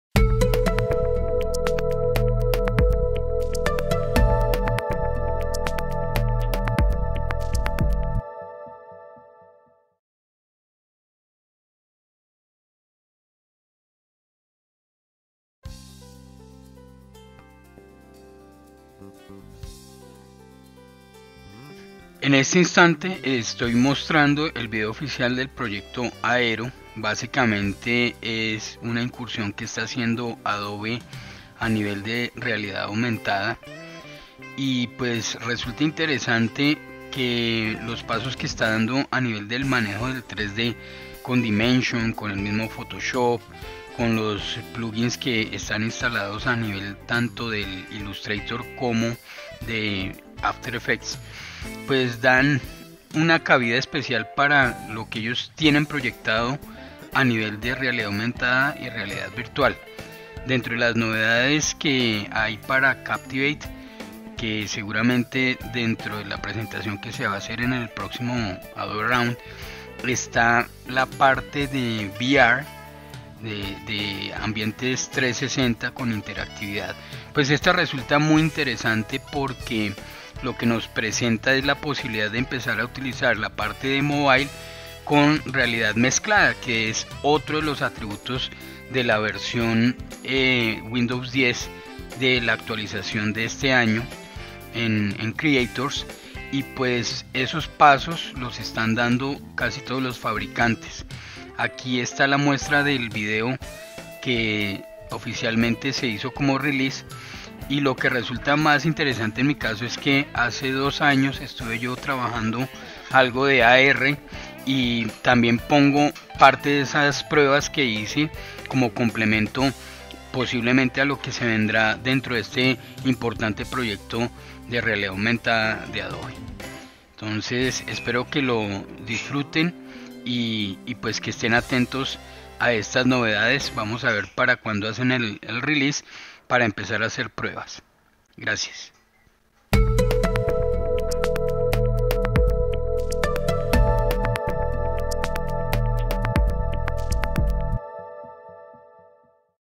Proyecto Adobe Aero AR 2018
Presentación del proyecto Beta Adobe Aero de Realidad Aumentada.
Algunos comentarios antes del Beta Release y seguramente de su presentación para el Adobe max de octubre de 2018.